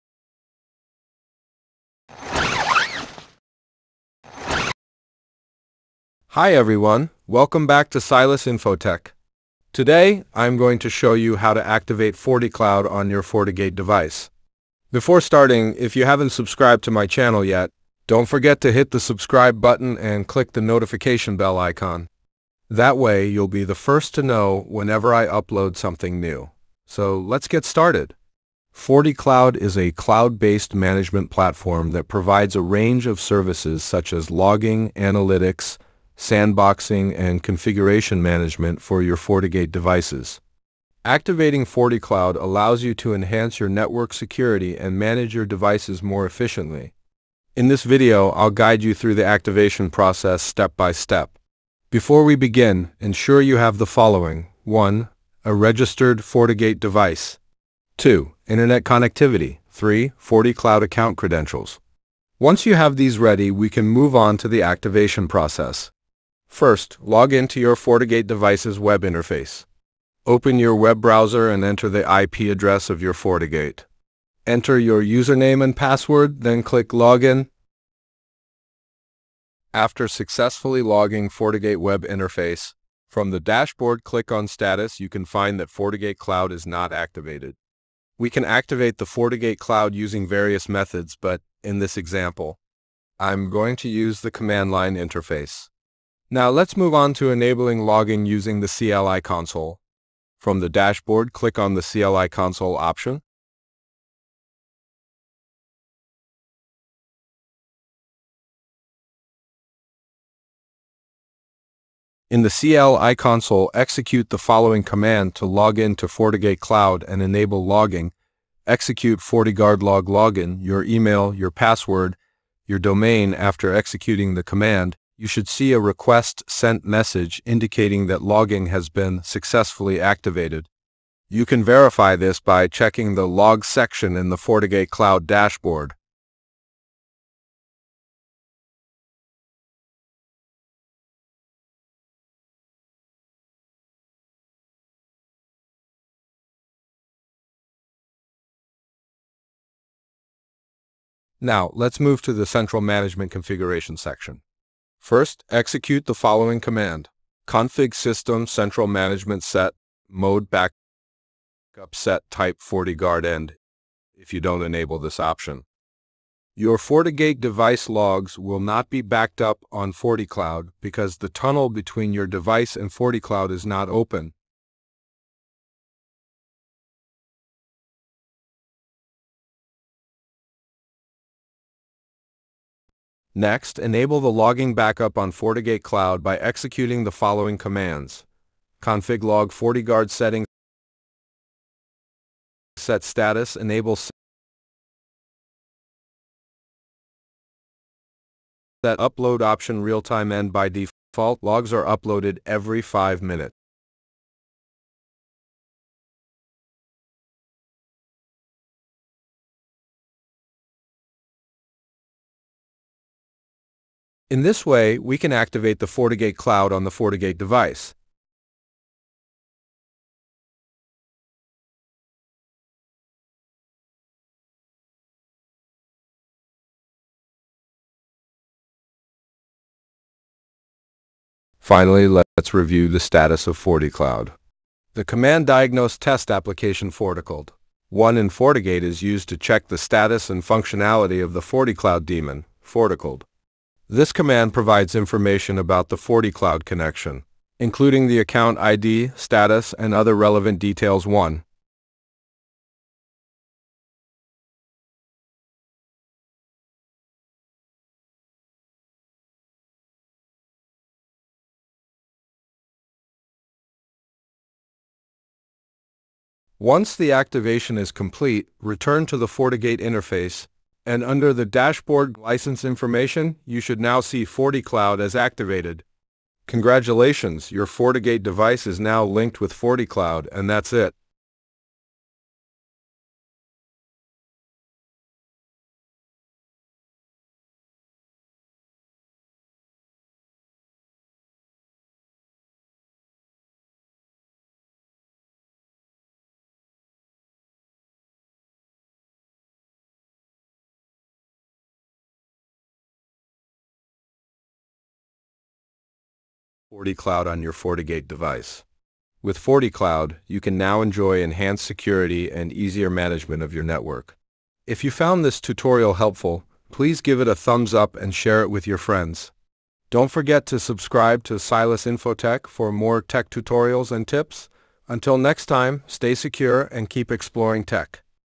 How to activate FortiGate Cloud from CLI & Enable Logging
FortiCloud is a cloud-based management platform that provides a range of services such as logging, analytics, sandboxing, and configuration management for your FortiGate devices. Activating FortiCloud allows you to enhance your network security and manage your devices more efficiently. In this video, I'll guide you through the activation process step-by-step.